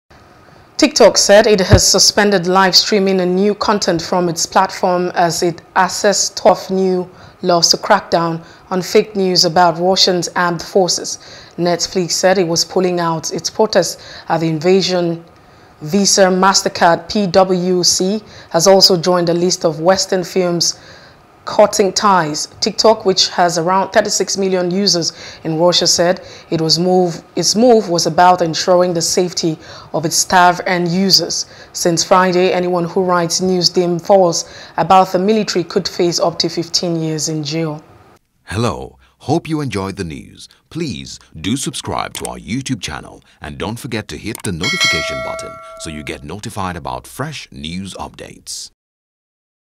Tiktok Limits Services As Netflix Pulls Out Of Russia | FOREIGN
TikTok said it had suspended live streaming and new content from its platform as it assesses tough new laws to crack down on "fake news" about Russia's armed forces.